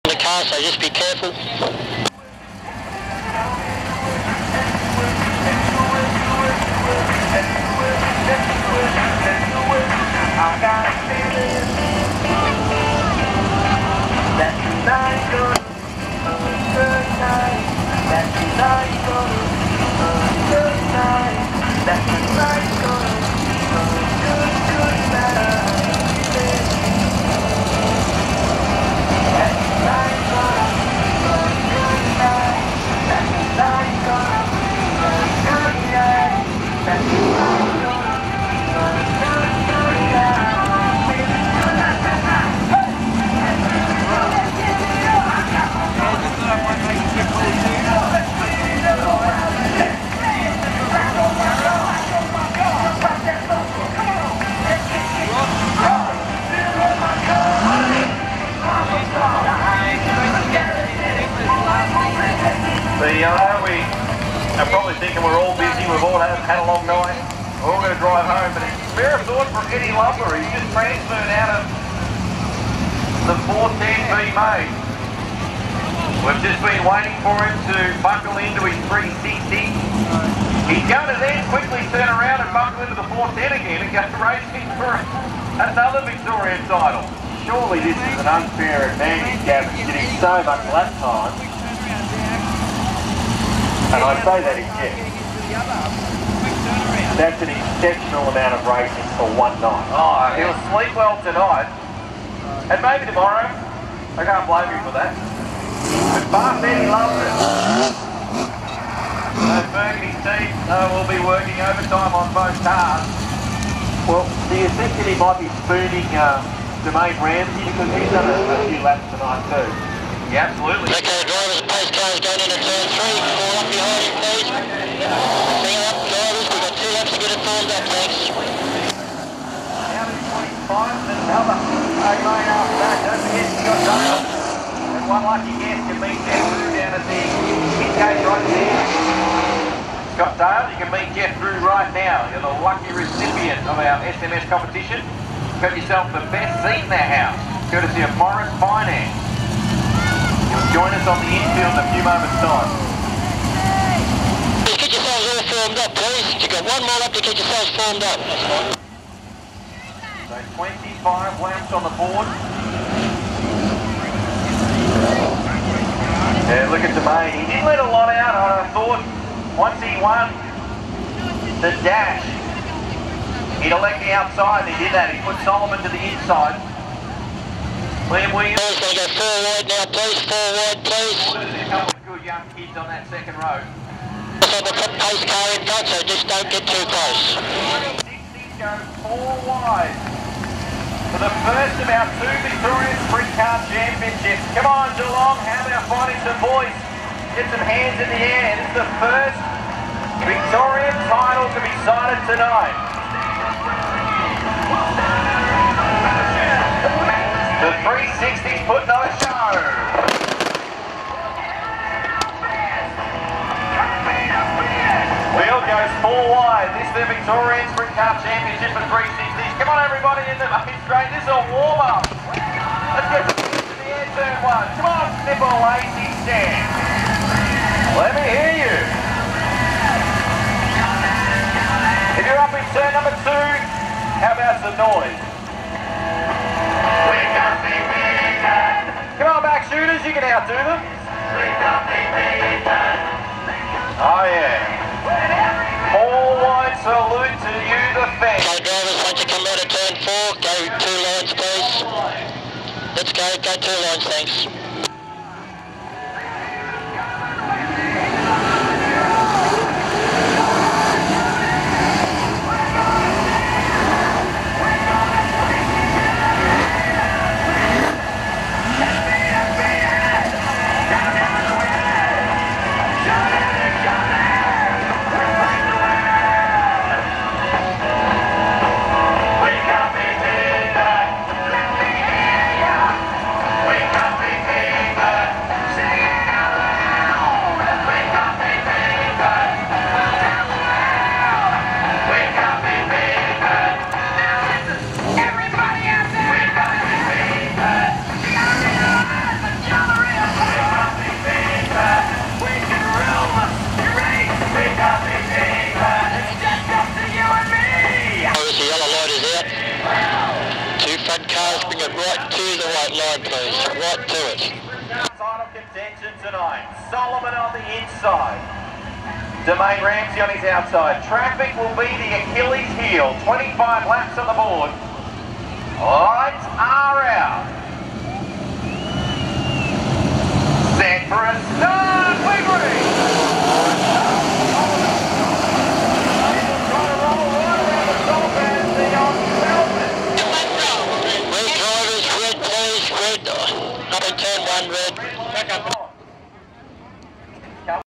Michael Cunningham "A" main for the Vic 360 title until a red light stoppage
Michael Cunningham "A" main for the Vic 360 title until a red light stoppage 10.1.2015.The voice you can hear is the race director talking to the drivers through their one way communication.